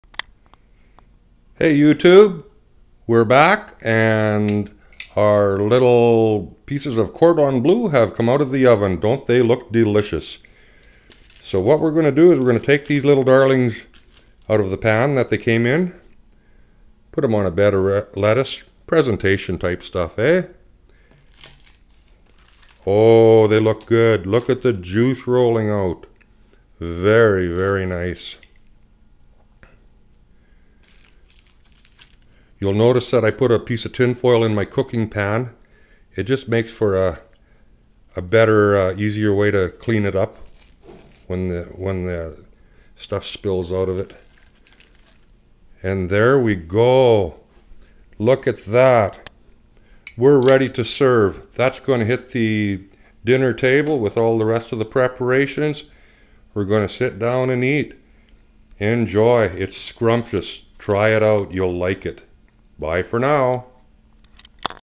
"Chicken Cordon Bleu" ....out of the oven Part #2
I just know you will enjoy this fabulous chicken dish. I also like to turn the chicken over part way through the baking proccess and brush the pieces with drawn butter. Yummm. Enjoy!
I had to upload "Cordon Bleu" video in two parts. For some reason I could not get my movie maker(editor) to compile into one vid.